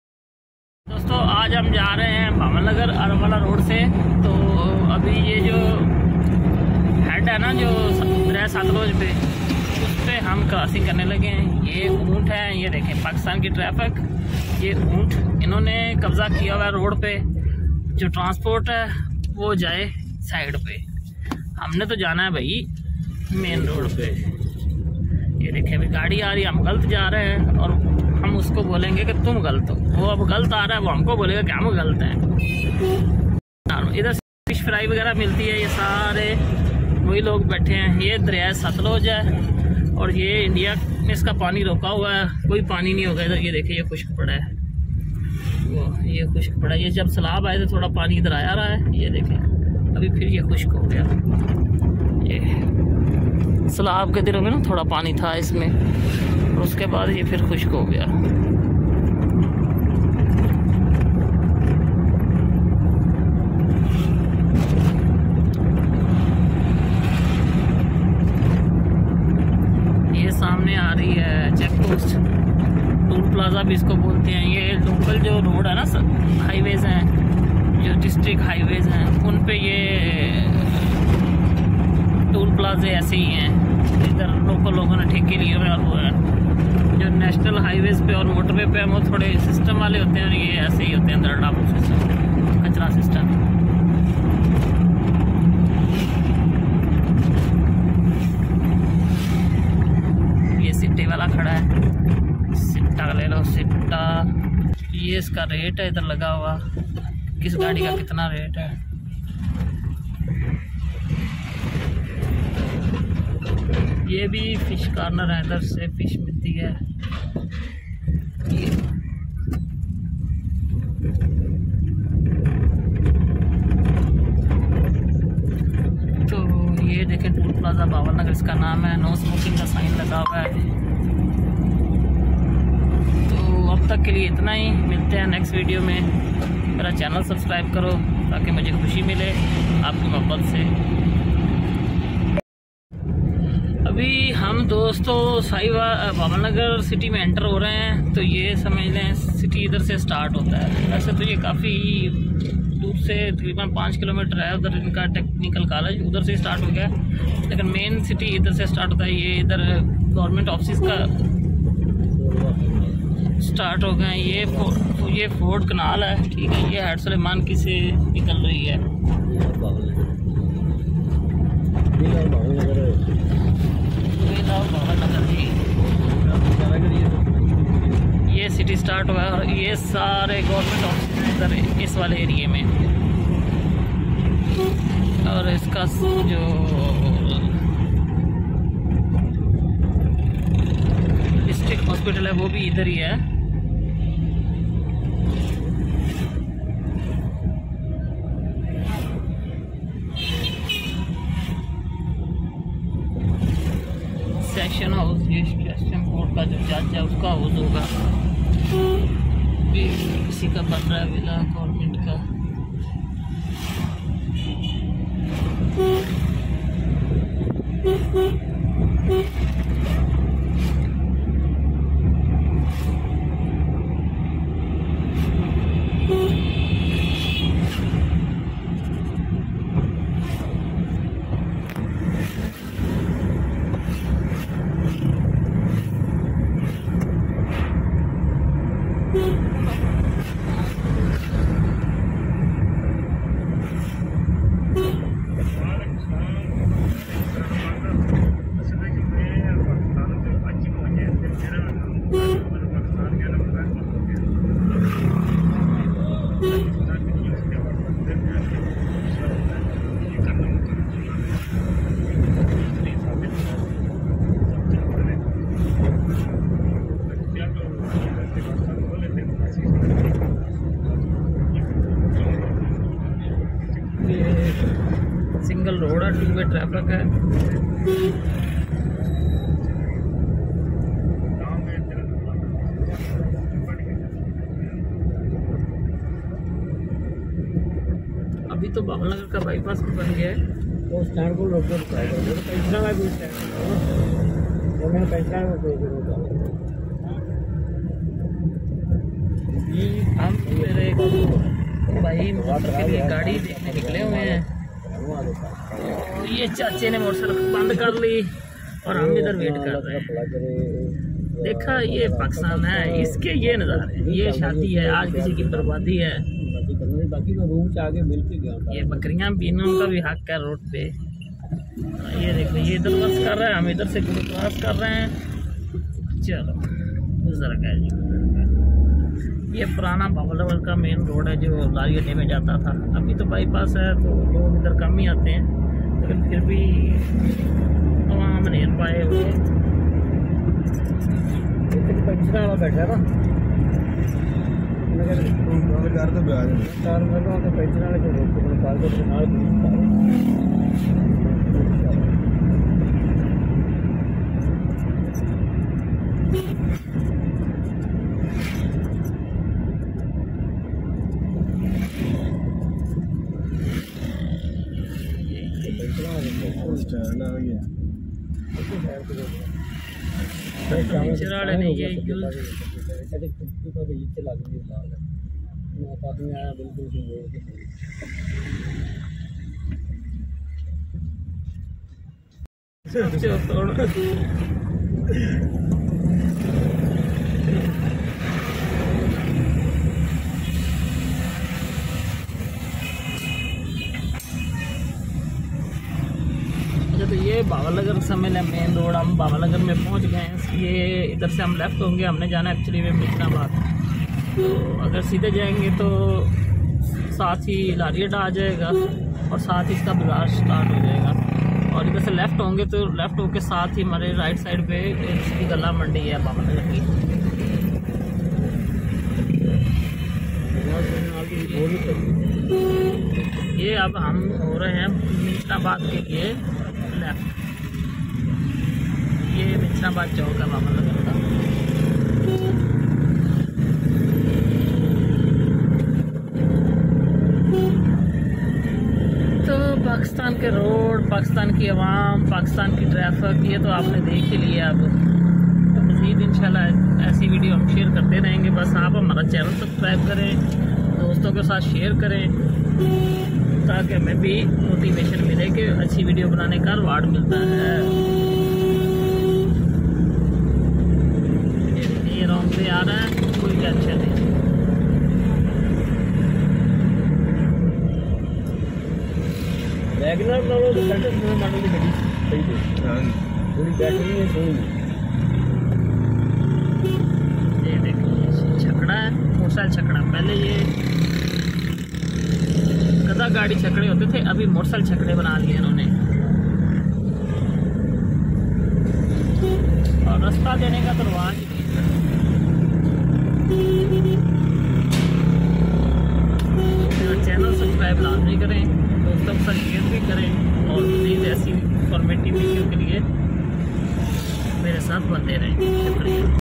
Bahawalnagar: City of Punjab Pakistan | Pakistan Roads and Traffic | Pakistani Highways
In this video you can watch Bahawalnagar a city of Punjab Pakistan virtual tour.
During this video you can watch road and traffic condtions of Bahawlanagar City and what are the key location you can visit using roads shown on video.

Bahāwalnagar
Bahawalnagar, Punjab, Pakistan